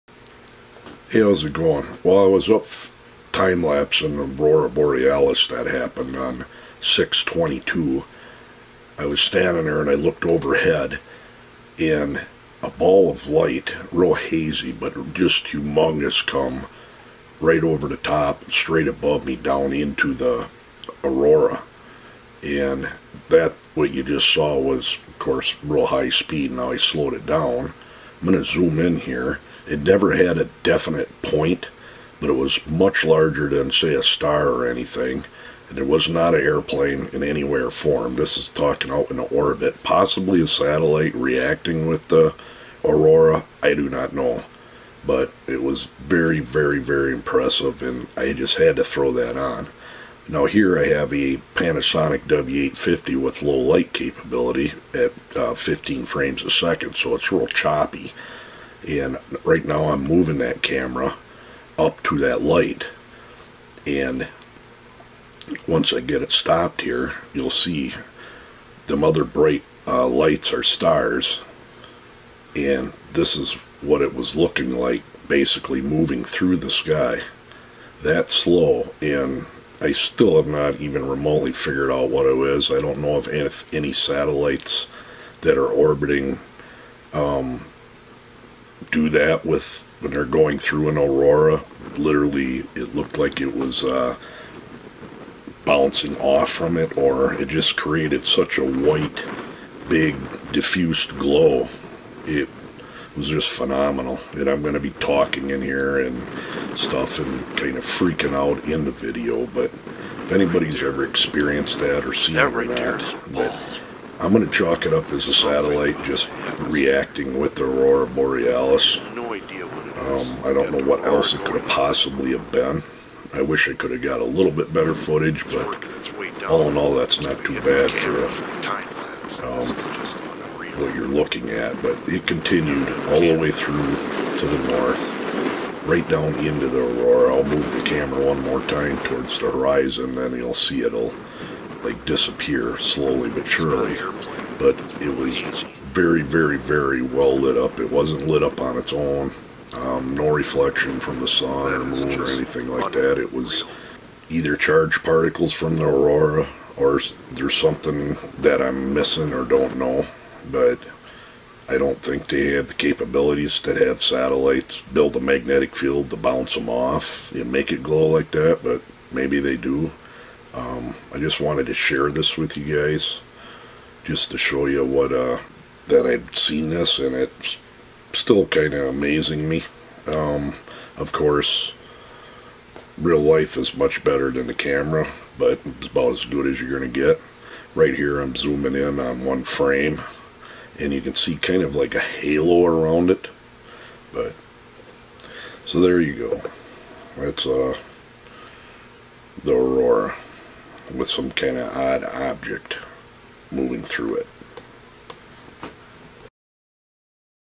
My weirdest Object that I have ever filmed in space
Witnessed a weird large object while filming and taking pictures of a very strong Aurora borealis
Pretty awesome  Possibly a Satellite reacting with the particles I ahve no idea
Too bad my camera sucked